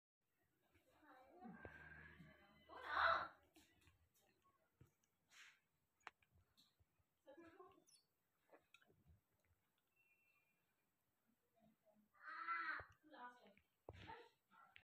Clean View Of Greyparrot Face
Grey Parrot Clear Face Zoomed View Must Watch & Take Knowledge About Greyparrot Beek Eyes & Ear